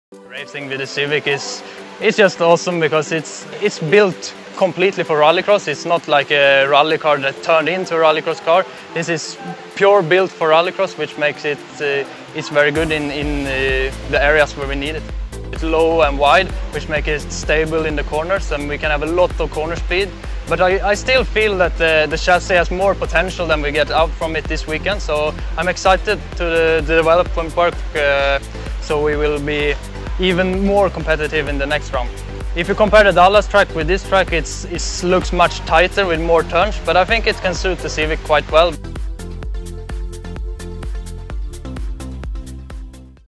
HPD Trackside -- Red Bull Global Rallycross: The Car
Want to know more about the Honda Civic in Global Rallycross? Ask Sebastian Eriksson, driver of the 93 car.